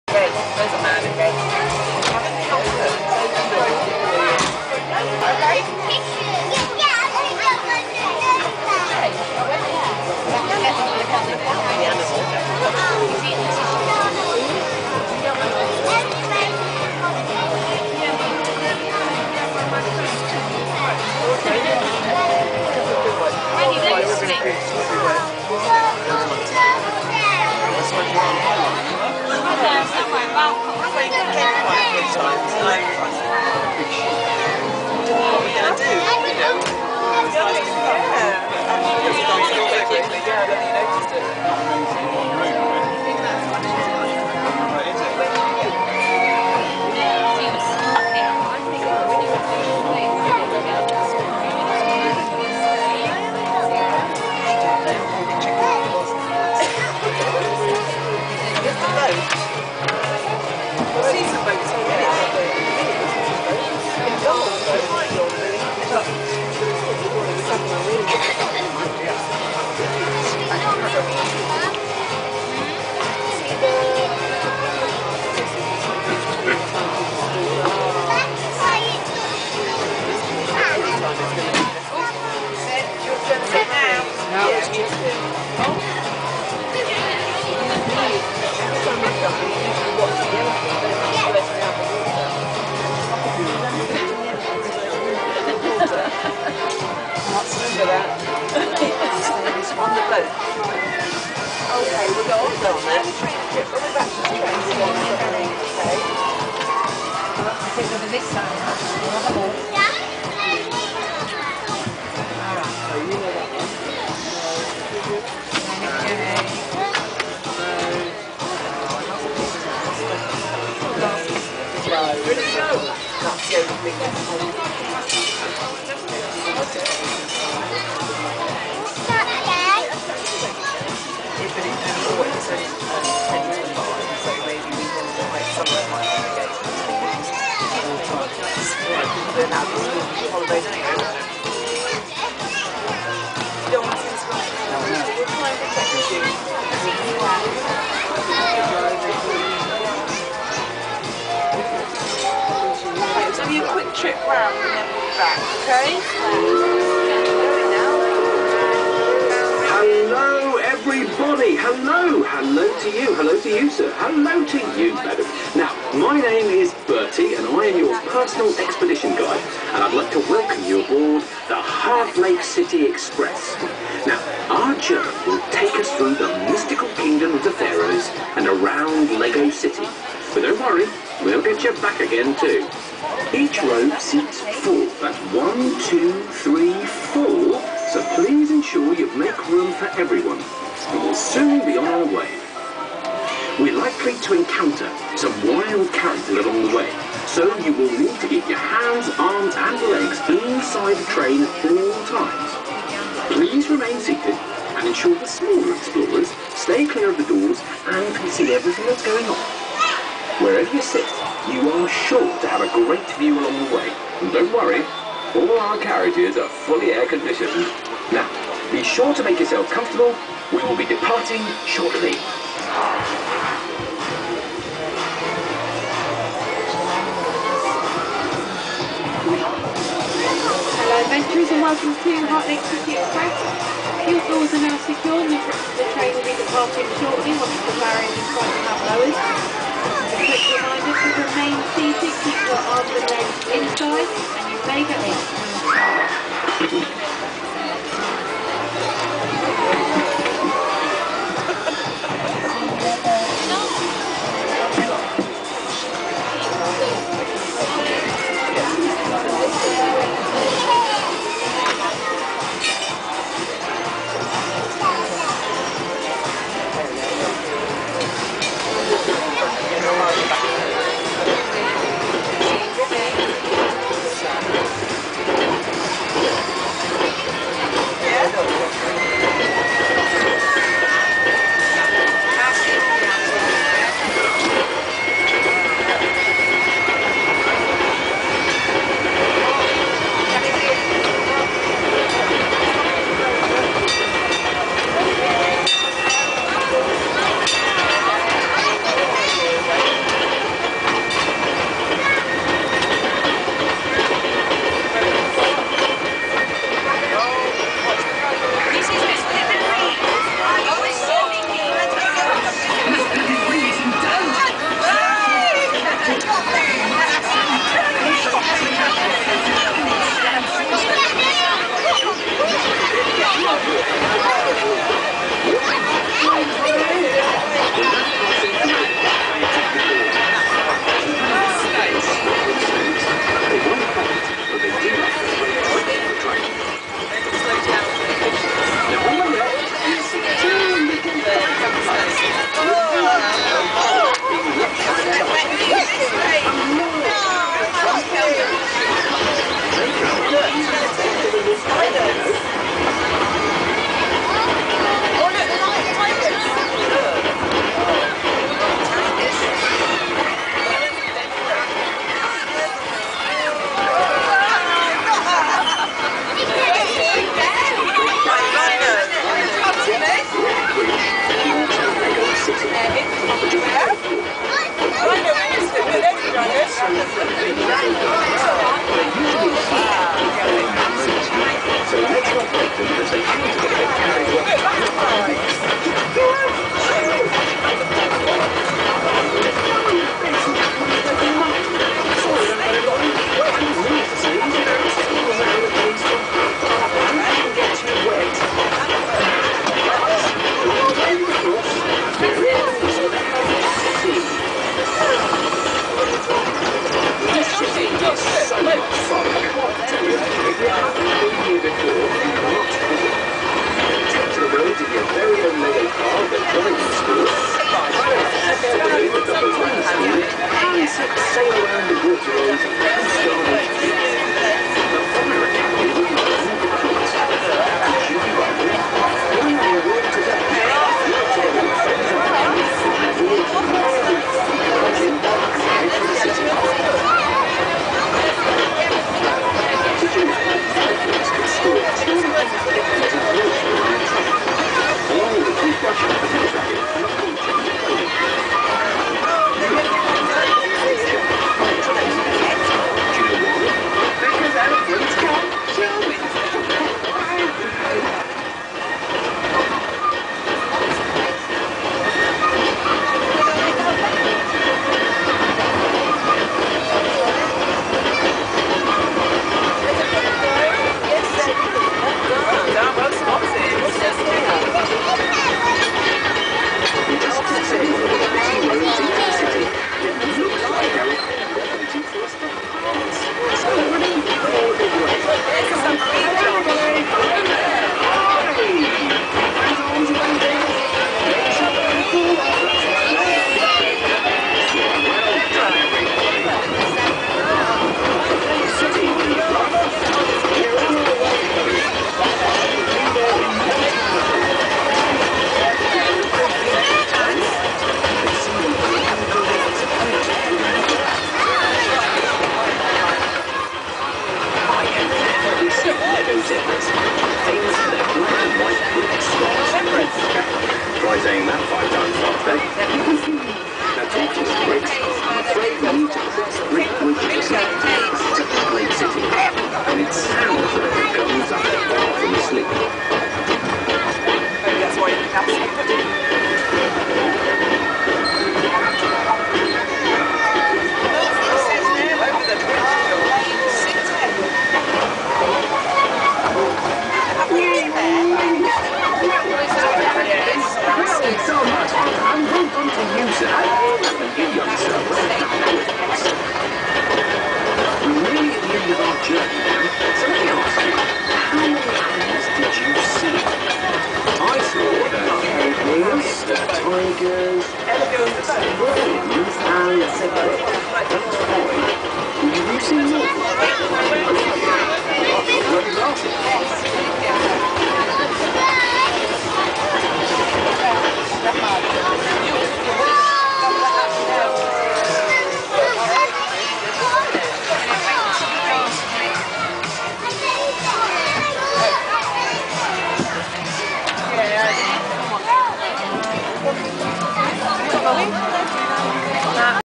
LEGOLAND Windsor Resort - Heartlake City Express 2016 (Full Ride)
I Chose to go on the Heartlake City Express Train Ride on Friday 11th March 2016.

Former names: I-Spy Express (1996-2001, Adventurers' Express (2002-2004) and Orient Expedition (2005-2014)

Manufactured by Severn Lamb in the 1990s and Entered Service in March 1996.